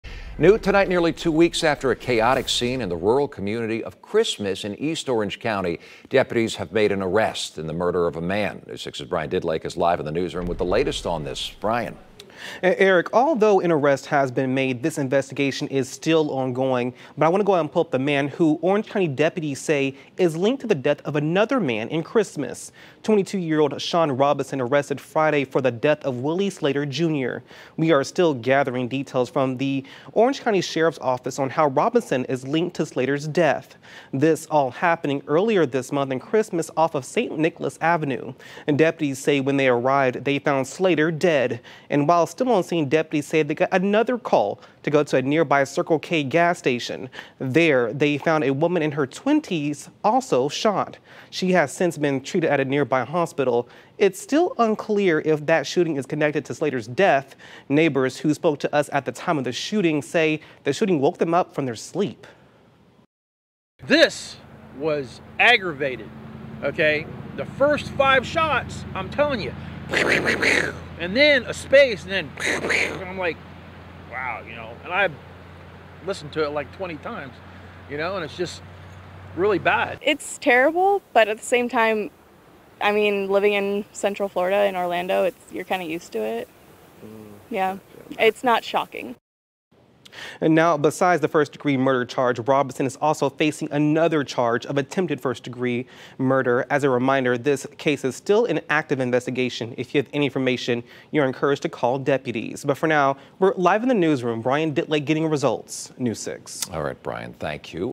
Deputies make an arrest after a murder in a rural community
An unsolved murder is solved